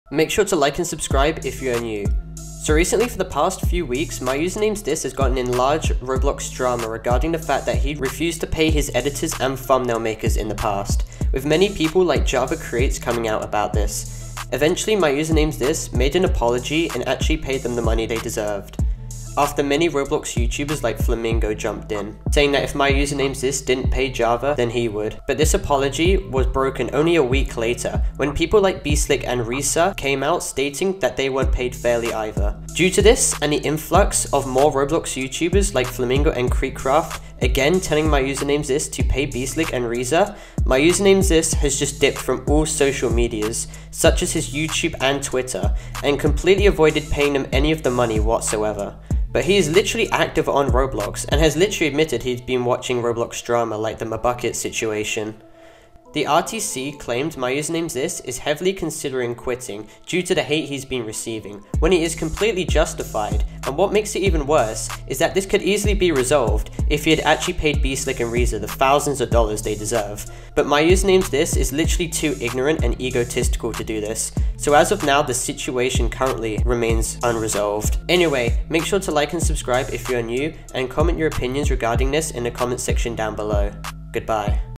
THIS ROBLOX YOUTUBER QUIT?... (Flamingo, KreekCraft & MyUsernamesThis DRAMA)
MyUsernamesthis QUIT youtube and twitter after this huge roblox drama happened...
Flamingo, KreekCraft & MyUsernamesThis DRAMA explained.
This was HUGE ROBLOX drama in the roblox community, with famous roblox youtubers such as Rainster, Flamingo and KreekCraft and the WHOLE RTC (Roblox twitter community) being mad at myusernamesthis